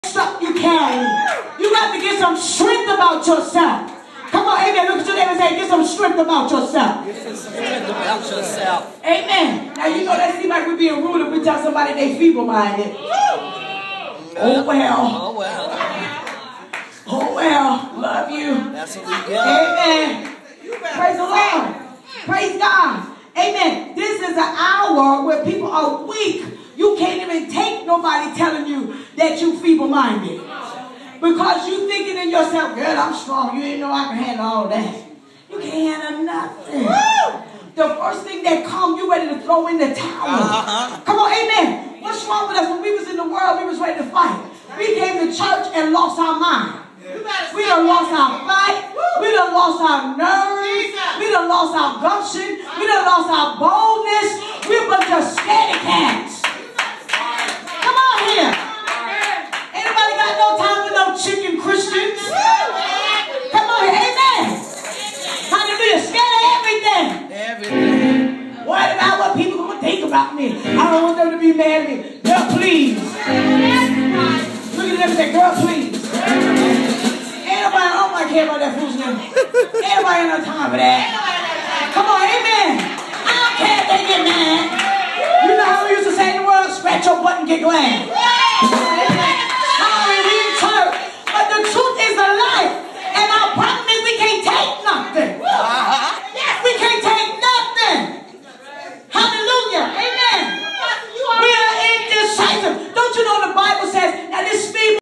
Silly Women Excerpt Pt 2
Apostle Fran continues preaching on this volatile subject!